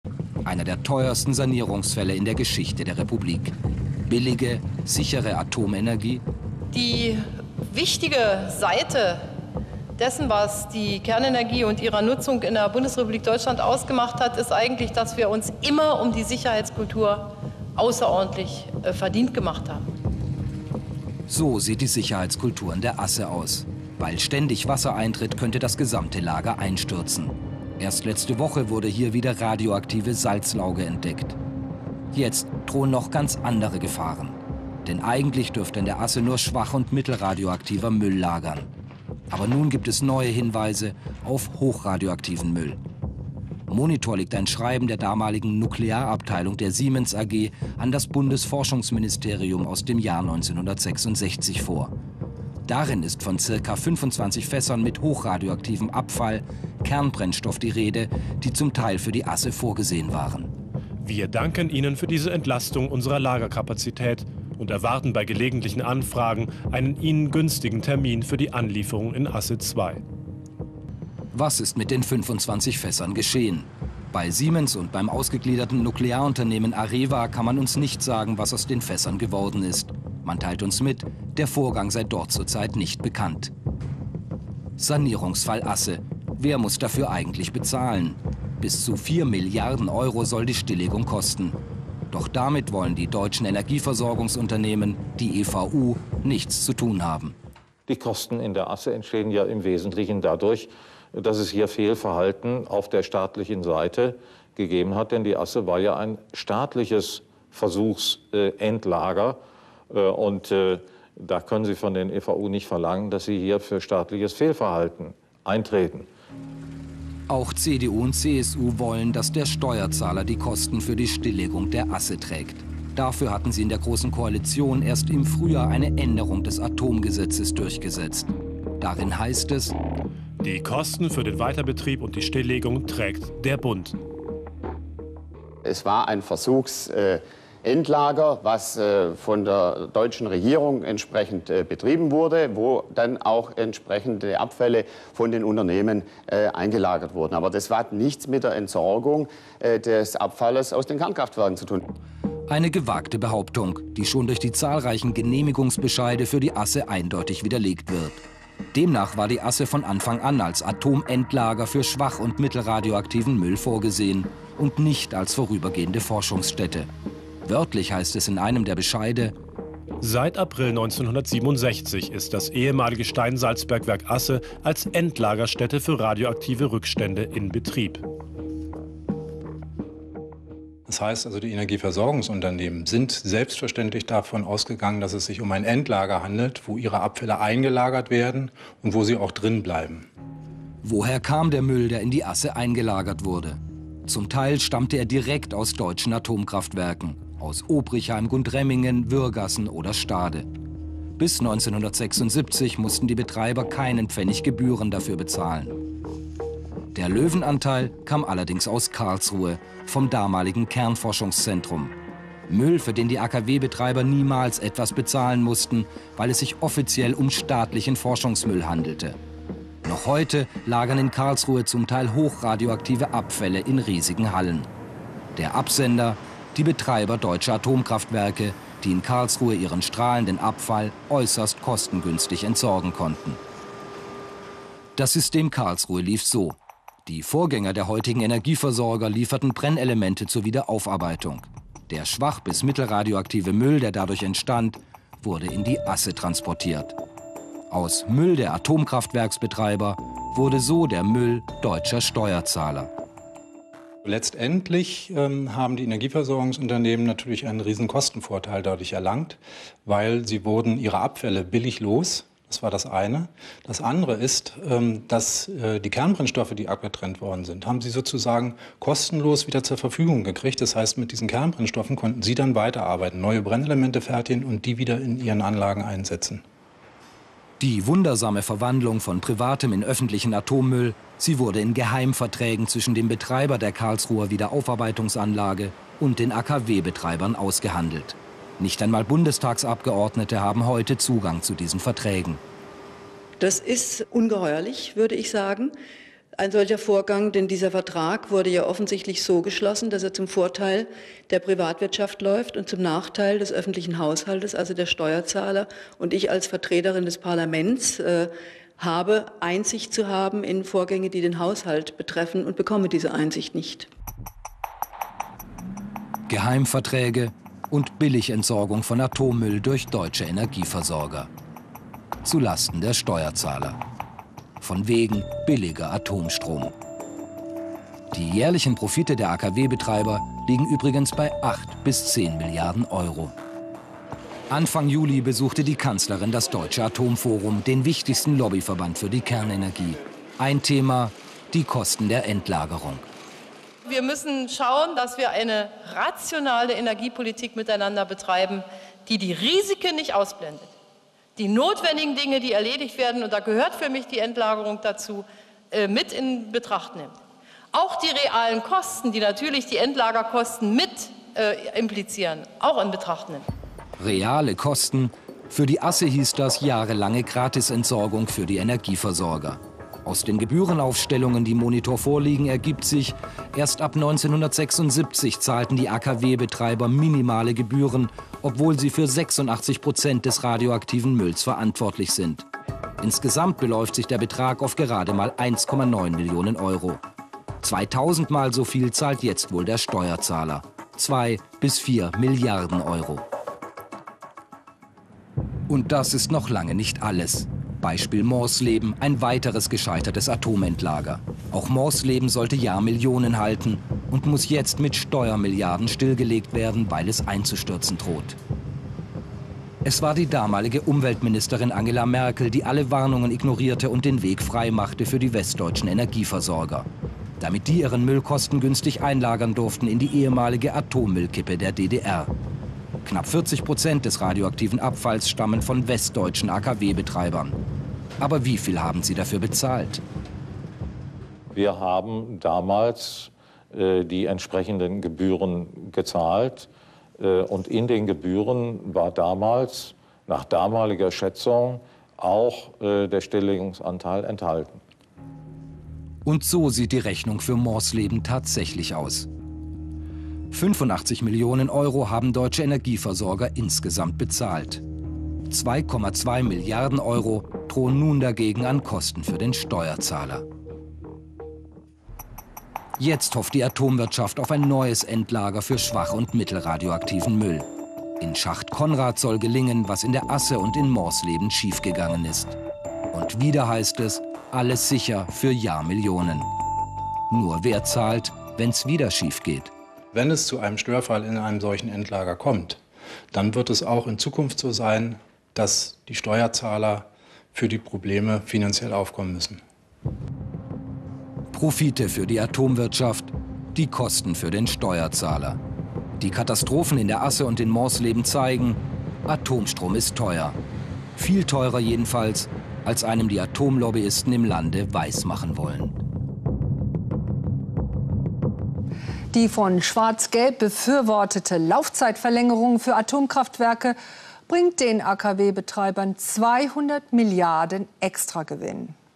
Atommülllager Asse - Korruption und Schlamperei
Atommülllager Asse - Katastrophe durch Schlamperei
Rechtswidrige Verträge - rechtsstaatswidriges Vorgehen
Alles im Interesse der Energiekonzerne - ohne Rücksicht auf das "Wohl des Volkes"